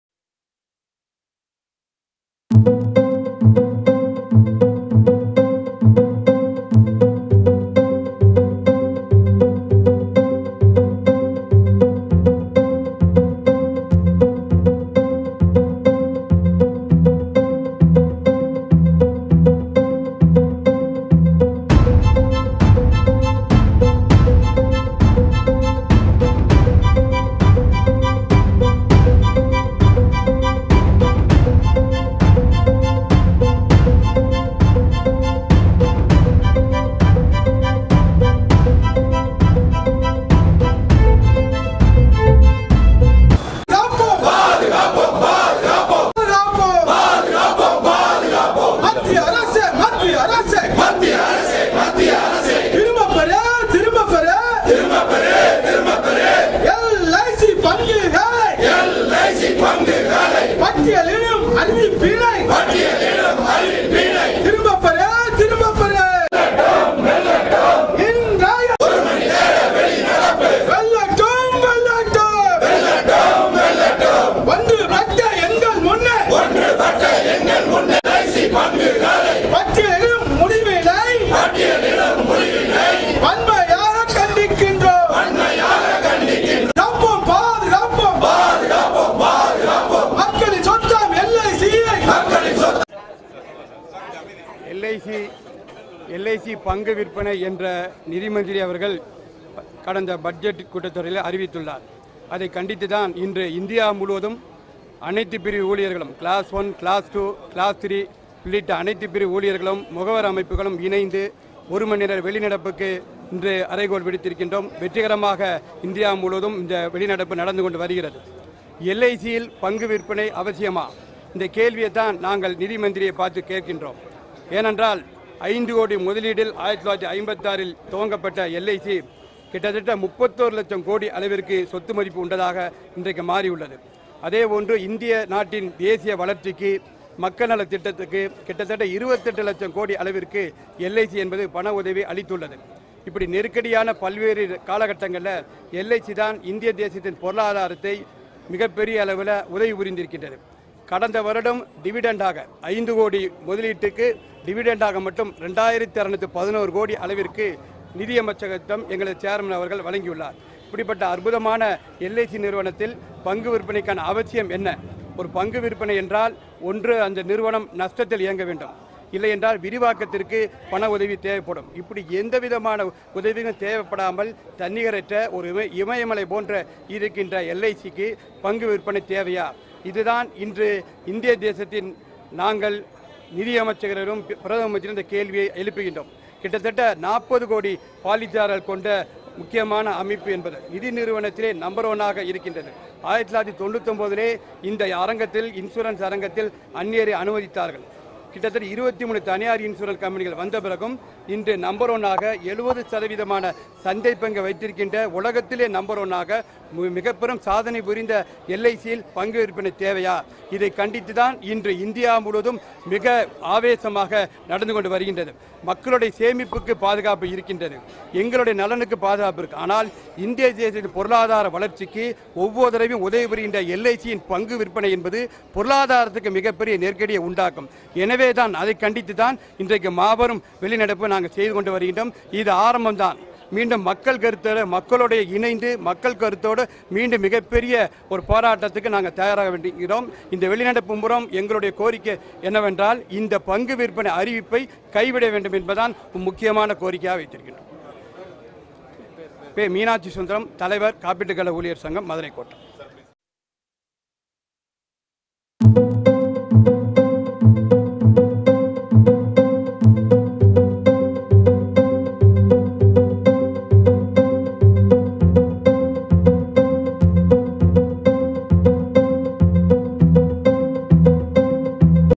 LIC Employees’ Unions Protest | Meenakshi Sundaram | Madurai | Sicp
Employees’ Unions Oppose LIC disinvestment Plan | Madurai | Sicp

Members of all employees’ unions to abstain from work for an hour today

Opposing the announcement to disinvest Life Insurance Corporation of India, members of All India Insurance Employees Association have threatened to intensify their agitation and drum up support from the public to stall the Centre’s move.

Terming the LIC an ‘Amudha Surabhi’ of Indian economy, the Association members said the LIC’s contribution to the 11th Five Year Plan (2012-17) was a whopping Rs. 14.23 lakh crore. In the 12th Five Year Plan, the Corporation had already given Rs. 7.01 lakh crore to the Centre in the first two years. On an average, the LIC’s annual contribution had risen from Rs. 2.84 lakh crore in the 11th Five Year Plan to Rs. 3.50 lakh crore in the 12th Five Year Plan.

The LIC’s assets stood at Rs. 32 lakh crore. The Corporation had invested in railways, national highways, port development, electricity, waterbodies and drinking water projects.

The LIC had also invested in many government bonds, the value of which stood at Rs. 28.84 lakh crore. Across the country, it had close to 12 lakh agents and policies to cover people from all walks of life.

At a time when the Centre was moving fast towards closing the PSUs and justified its decisions to privatise them, the LIC was nowhere in the picture as it was no match to private players in insurance sector.

The Association members on Saturday held a demonstration in the city, where the speakers said in the last fiscal alone, the LIC had given a dividend cheque for Rs. 2,611 crore to the Centre.

That the Corporation, which was started with a capital of Rs. 5 crore in 1956, never needed government’s assistance till date was a proof of its administrative abilities. The announcement in this Budget by Finance Minister Nirmala Sitharaman had come as a big blow to millions of people across the country, they said.

The government should withdraw its announcement, failing which the employees would have to intensify their stir against the government, said Association general secretary N.P. Ramesh Kannan.

The Association members criticised the entry of foreign players in insurance sector and their exit after making good of their capital in a decade.

Armed with 40 crore policy holders, the LIC had the commitment of security and safety, they noted.

As a mark of protest, members of all employees’ unions across the country would abstain from work for an hour on Monday, Mr. Ramesh Kannan said.